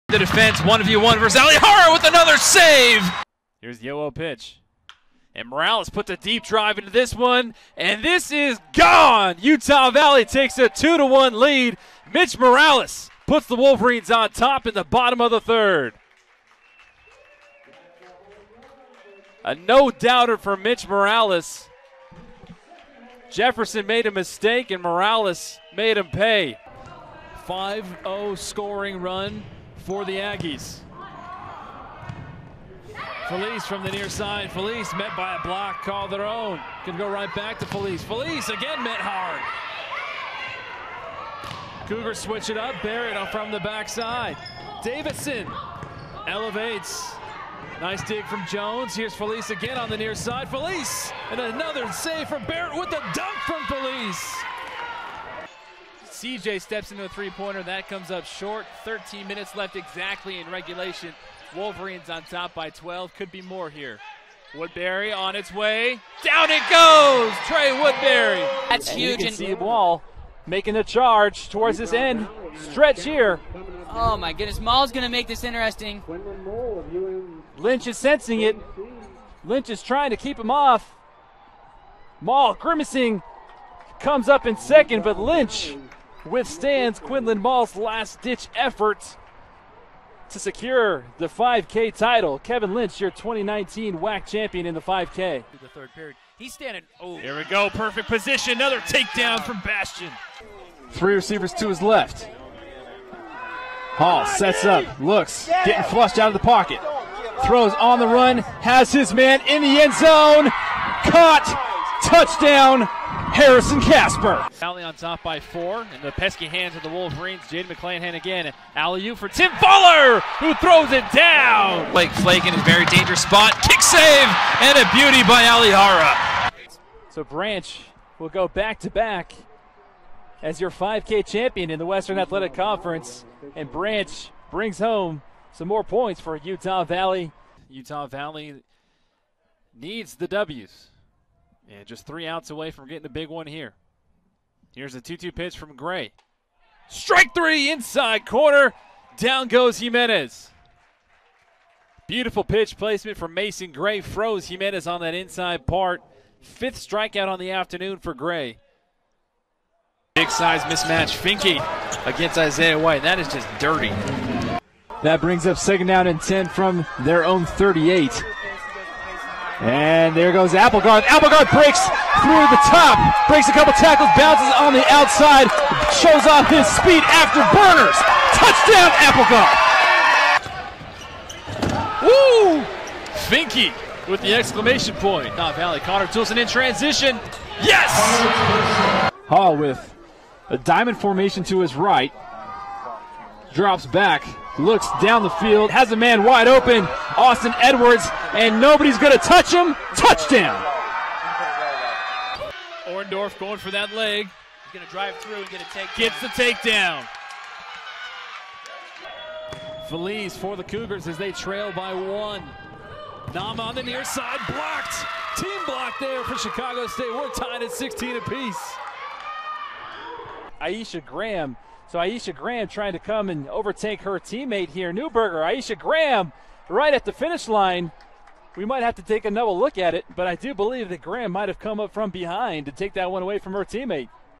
Brandon Crow multi-sport play by play reel 08.21
Clips and highlights of various sports I have announced at the D-1 collegiate level.